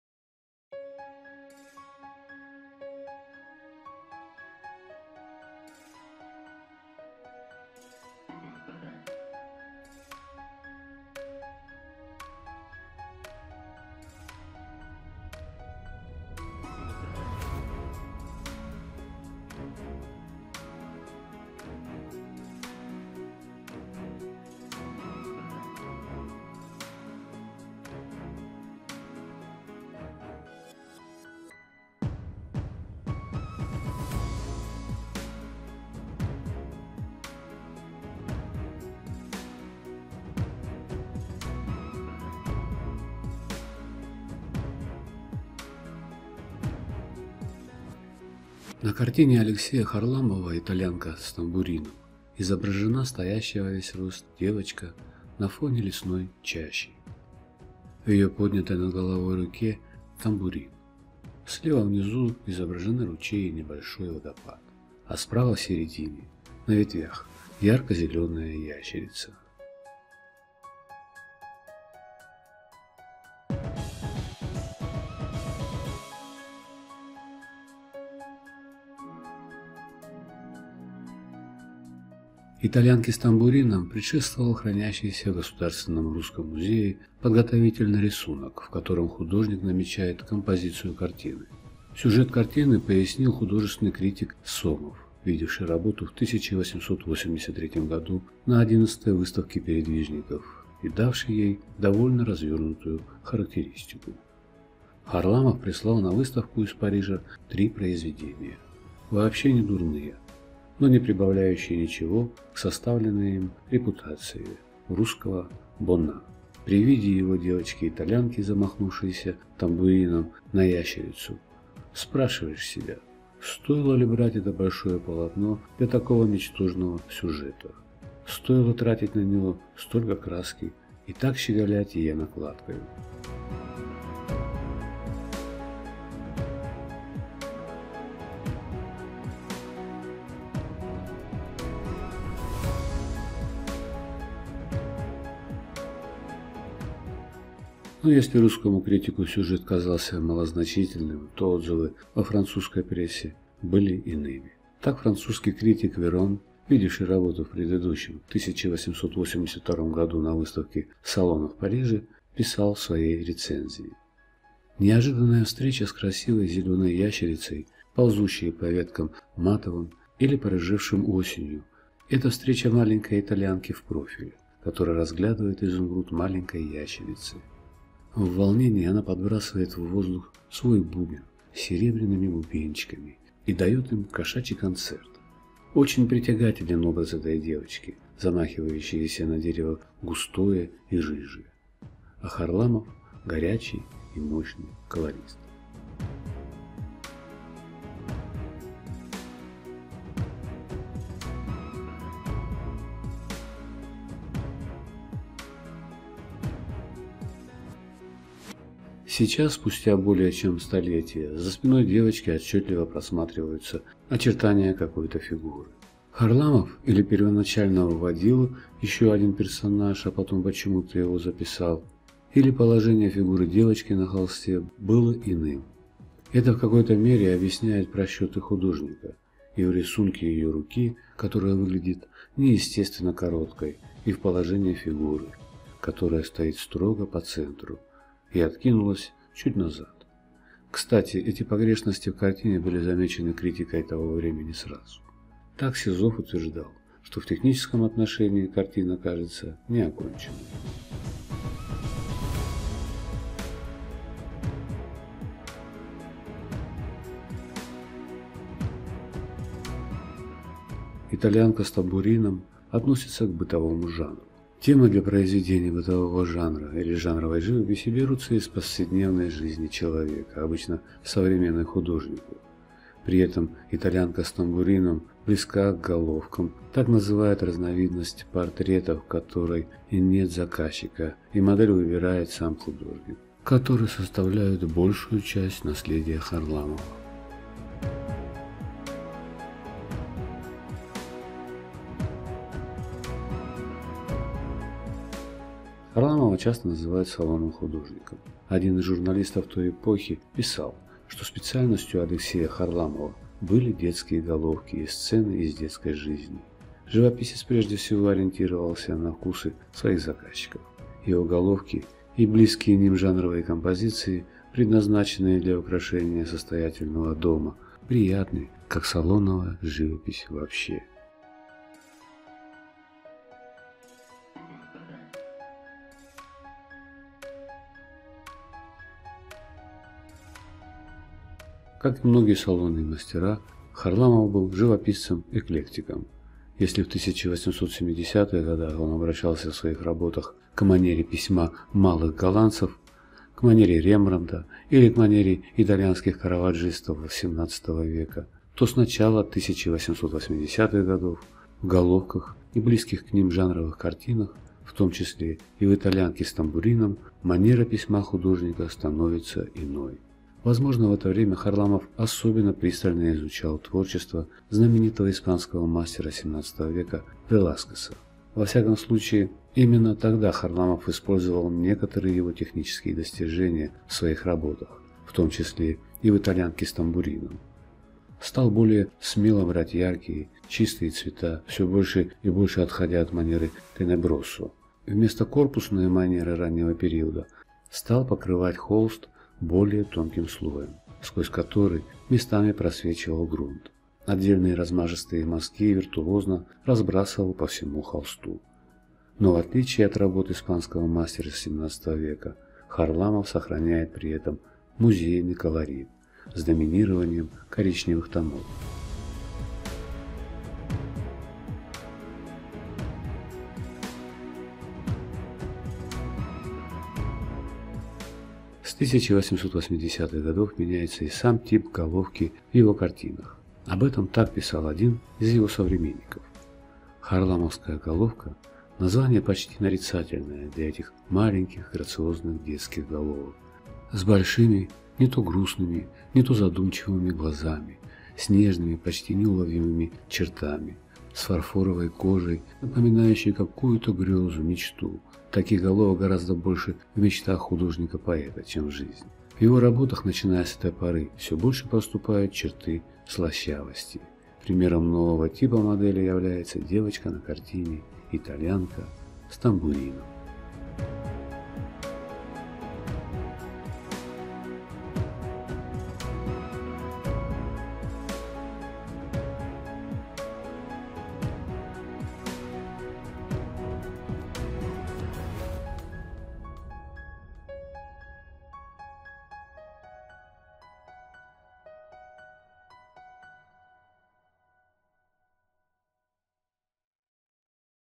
Алексей Харламов. Итальянка с тамбурином
На картине Алексея Харламова "Итальянка с тамбурином"
изображена стоящая во весь рост девочка на фоне лесной чащи.
В ее поднятой над головой руке - тамбурин.

00:00 Саратовский художественный музей имени А. Н. Радищева, Саратов

01:00 Описание картины:
Итальянка с тамбурином
Алексей Харламов
Портрет, Жанровая сцена
Стиль: Реализм
Дата 1882 г
холст, масло
Размеры
Местонахождение картины Итальянка с тамбурином:
Радищевский музей

01:43 Рисунок - итальянка с тамбурином
Год 1970-е
Бумага, тушь
Размер: 33,5×20,5 см.
Работа представляет собой подготовительный рисунок для
итальянской девочки А. Харламова с бубном из Саратовского
государственного музея изобразительных искусств им. Радишева,
который был выставлен на парижском Салоне 1882 года.